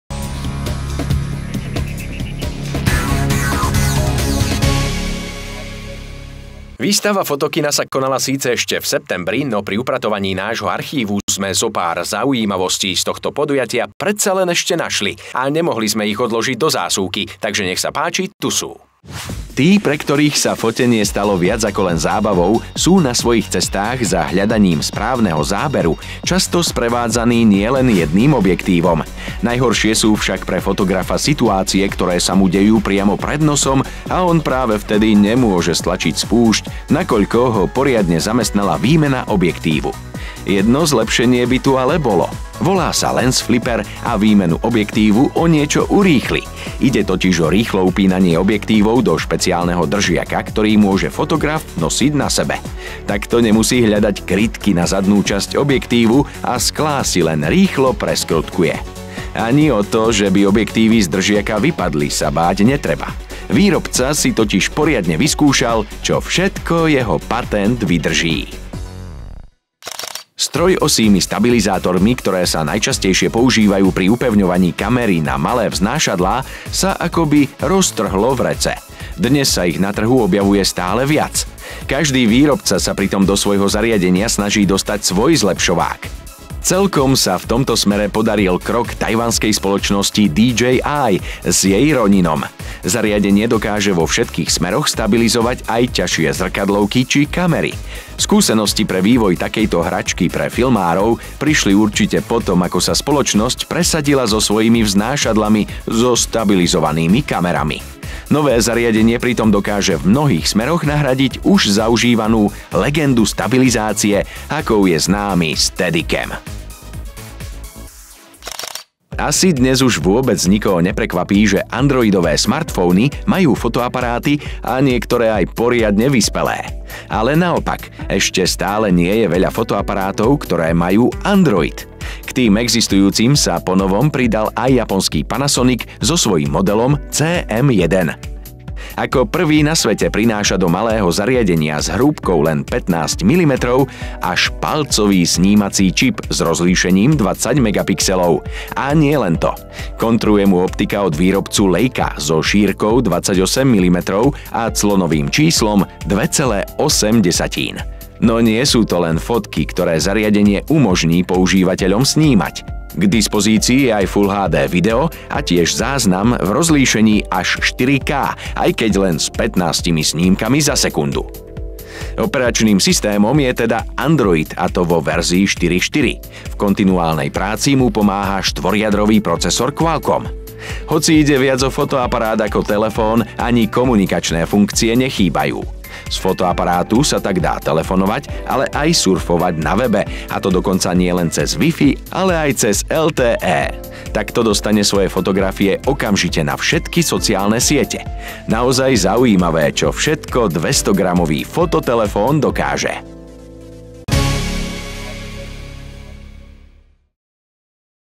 Photokina 2014: Lens Flipper, DJI Ronin, Panasonic CM1
Photokina 2014 a jej zaujímavosti zo sveta filmárov a fotografov. Viac na